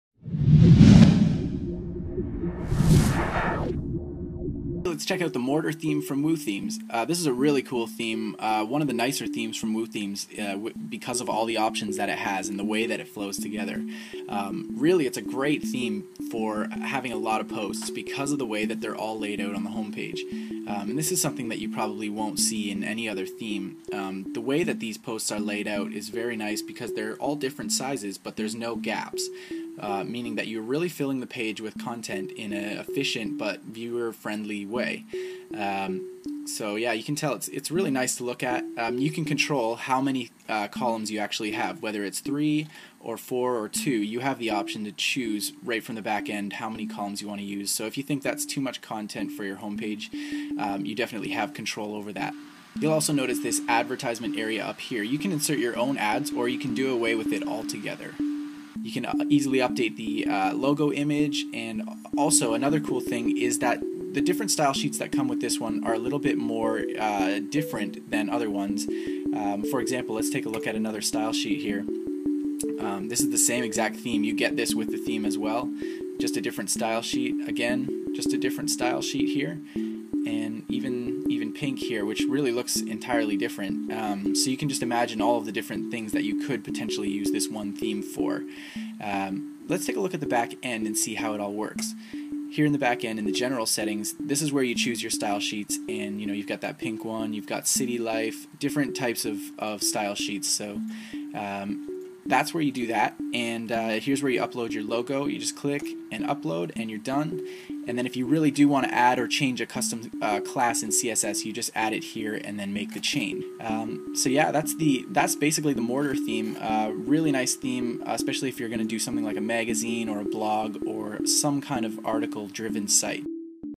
ThemeCouponCodes.com Theme Review - Mortar Theme from Woothemes
The Mortar Theme from Woothemes is a beautiful theme for Wordpress. It allows content to be displayed in a organized but effective way by filling the page with posts in a grid layout. Great for Online Magazines, Blogs.